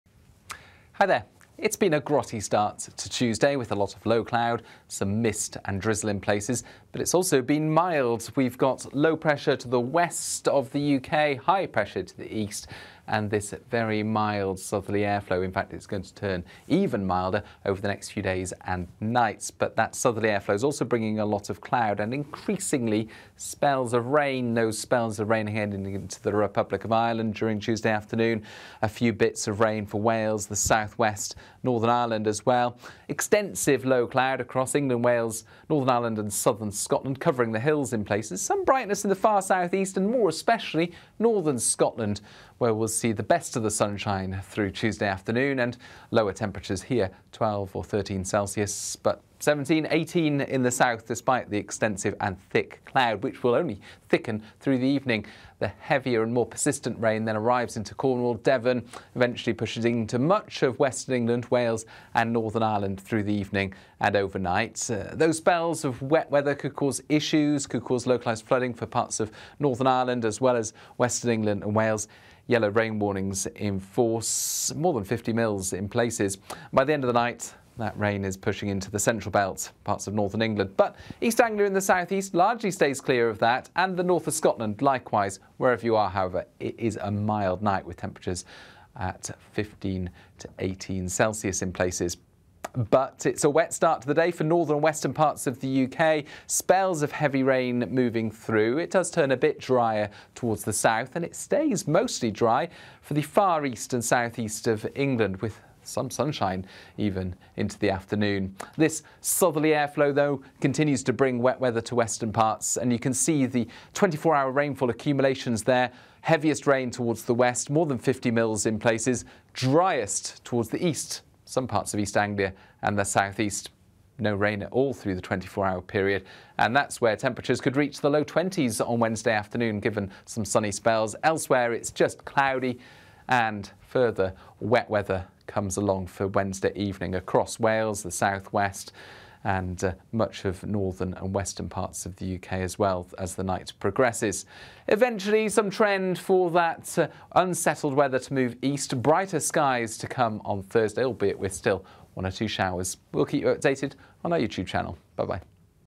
15/10/24 – Cloudy for most – Afternoon Weather Forecast UK –Met Office Weather
Southerly airflow continues to bring mild conditions. Low cloud covers most of the UK with areas of mist, drizzle and patches of rain, particularly over higher ground. Some clear skies in the far southeast of England and northern Scotland, giving bright and sunny spells. Cloud thickening throughout the afternoon, and remaining into the evening as we see the onset of bands of rain approaching from the southwest, tracking northeasterly throughout the evening to affect much of southern and central England, Wales and Northern Ireland. - This is the Met Office UK Weather forecast for the afternoon of 15/10/24. Bringing you today’s weather forecast is Aidan McGivern.